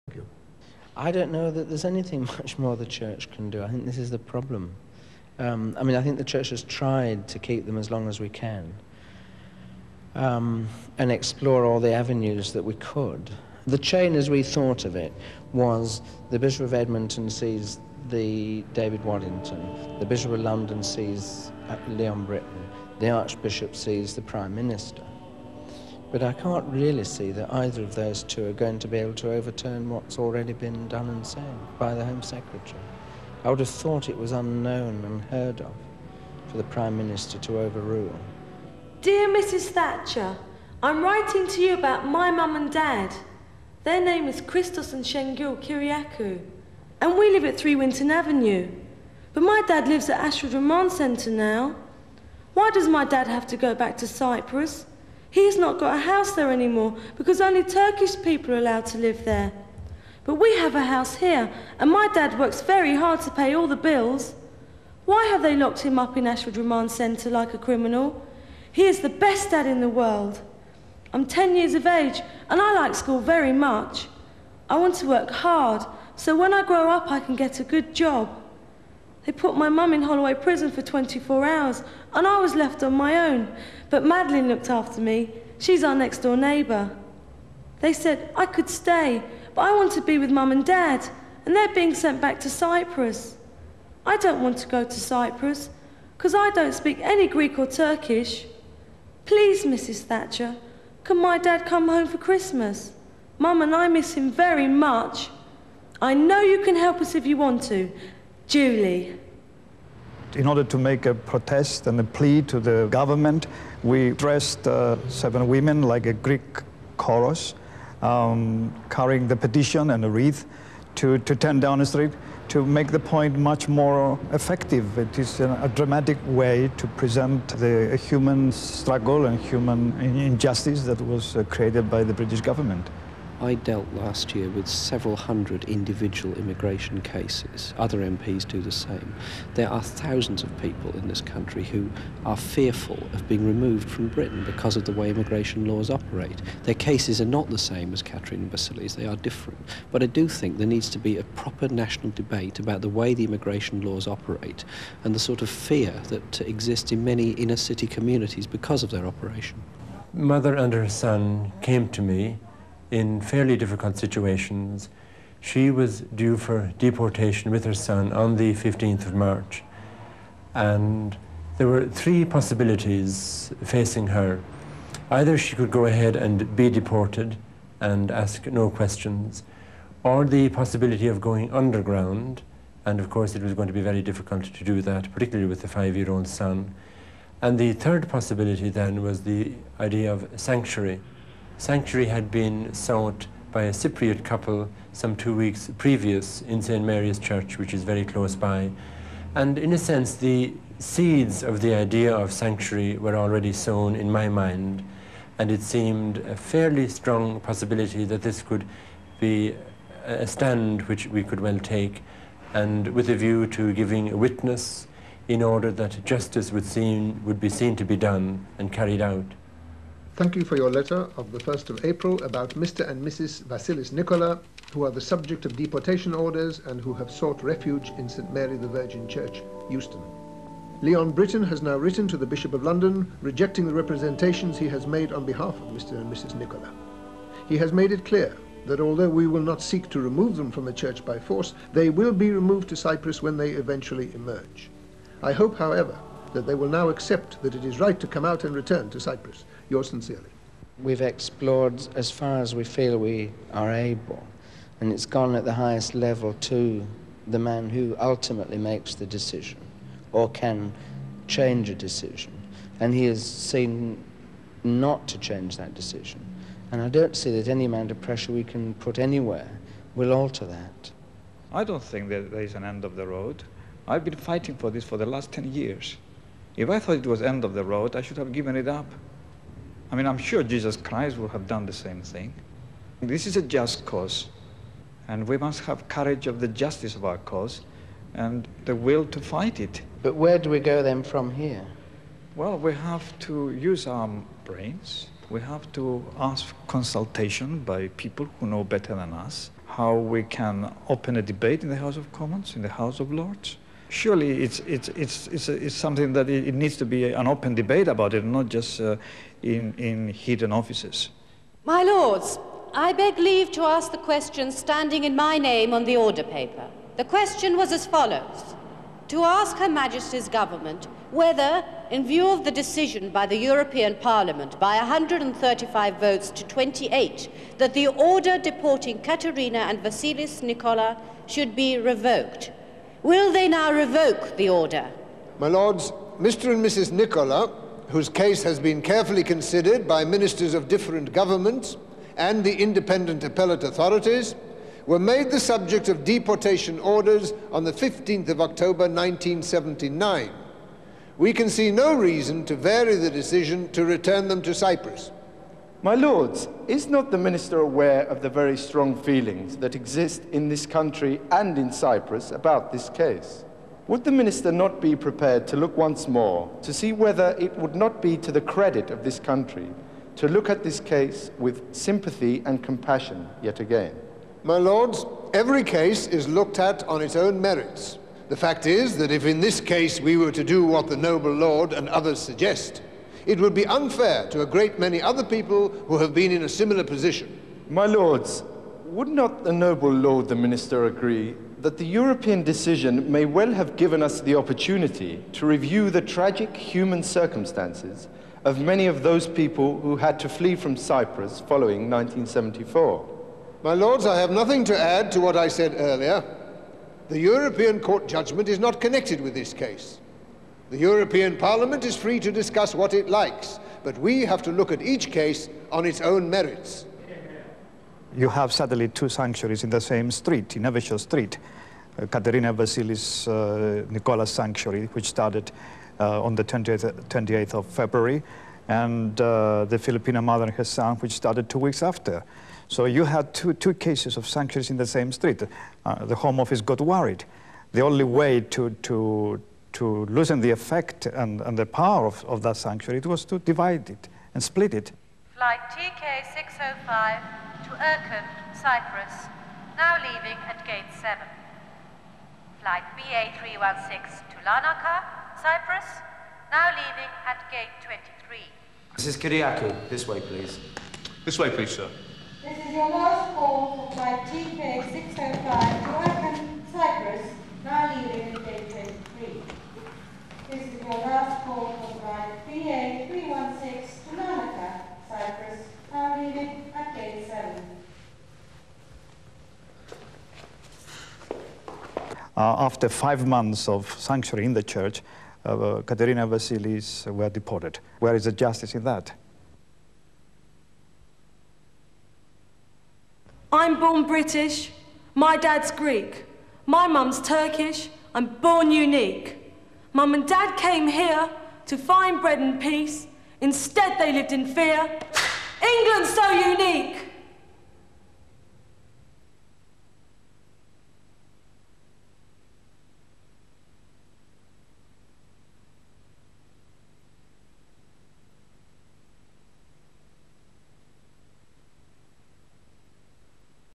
Sanctuary Challenge by Theatro Technis - 3of3
Intercuts scenes from the play "The Appellants" by George Eugeniou, based on the case histories of Greek Cypriot refugees and proposing sanctuary as a means of avoiding their deportation, with documentary footage of the case of Katerina and Vassilis Nicola seeking Sanctuary in St. Mary's Church in Camden during the Thatcher Government. The couple, who had been living and working in London since 1976, sought sanctuary during February 1985, and the case went on appeal to the House of Lords and was the subject of a vote by the European Parliament, but the couple were styill deported. This case was the catalyst for many other cases of refugees taking asylum in Churches during subsequent years.